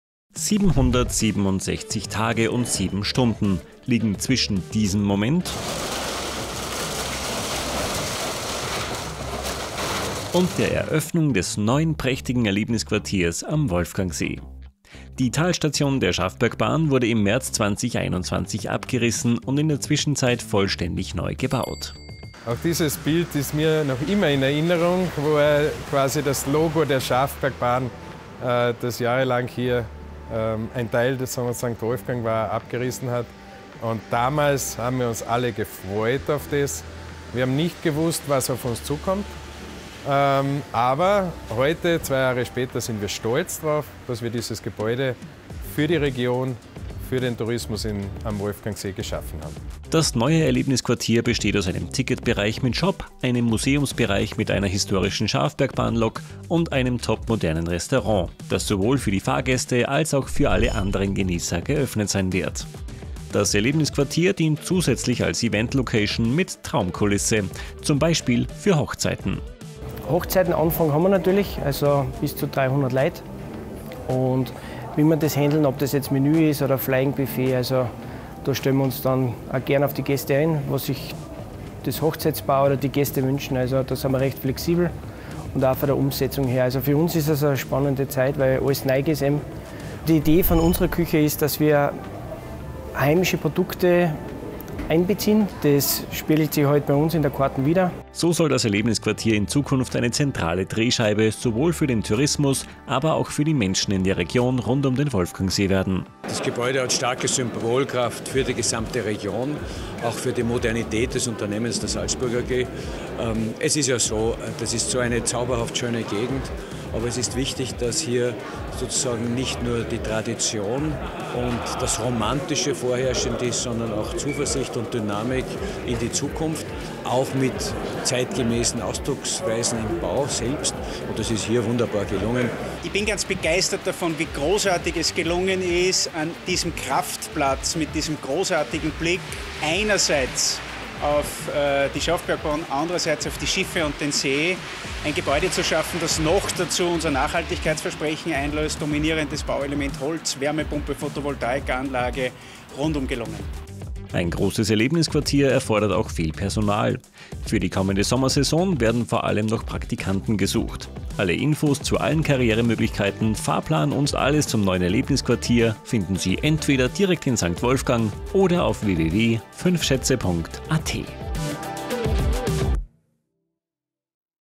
Schafbergbahn jubelt - Erlebnisquartier am Wolfgangsee ist eröffnet!
Die Schafbergbahn jubelt und die Wolfgangsee Schifffahrt freut sich mit, denn beide Attraktionen haben ab sofort ein neues Zuhause. Die Talstation der Schafbergbahn wurde in den vergangenen Jahren zum regelrechten Erlebnisquartier, jetzt ist es feierlich eröffnet! Neben dem Fahrkarten Service, bietet das Erlebnisquartier auch einen Shop, einen Museumsbereich mit einer historischen Lok der Schafbergbahn aus dem Jahr 1893 und ein top modernes Restaurant. Das Erlebnisquartier kann außerdem als Eventlocation gebucht werden, zum Beispiel um Hochzeiten dort zu feiern.

Die Schafbergbahn startet heuer am 29. April in die Sommersaison und freut sich auf zahlreiche Gäste. Infos zum Erlebnisquartier und zu Jobs rund um den Wolfgangsee finden Sie